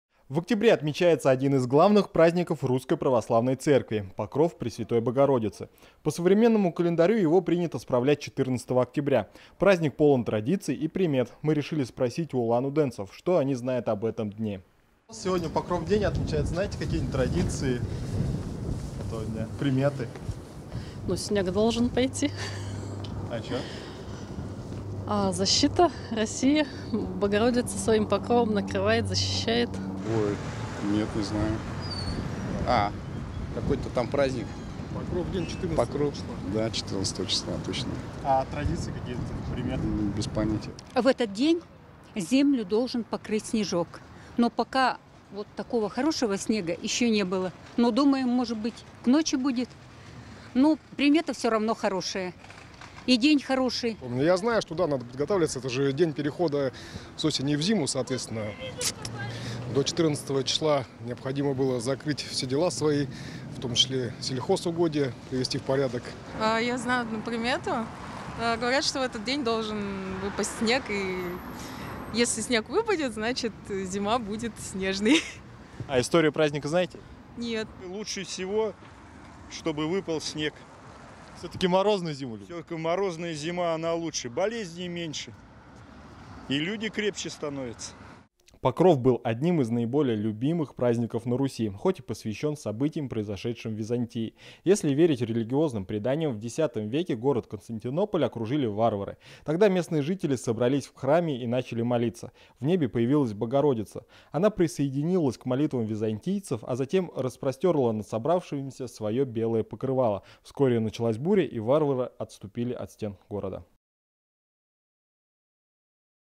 Сегодня россияне празднуют Покров Пресвятой Богородицы. Верят ли улан-удэнцы в приметы?
В октябре отмечается один из главных праздников Русской православной церкви - Покров Пресвятой Богородицы. По современному календарю его принято справлять 14 октября. Праздник полон традиций и примет. Мы решили спросить в улан-удэнцев, что они знают об этом дне.

Покров - был одним из наиболее любимых праздников на Руси, хоть и посвящен событиям, произошедшим в Византии. Если верить религиозным преданиям, в X веке город Константинополь окружили варвары. Тогда местные жители собрались в храме и начали молиться. В небе появилась Богородица. Она присоединилась к молитвам византийцев, а затем распростерла над собравшимися свое белое покрывало. Вскоре началась буря, и варвары отступили от стен города.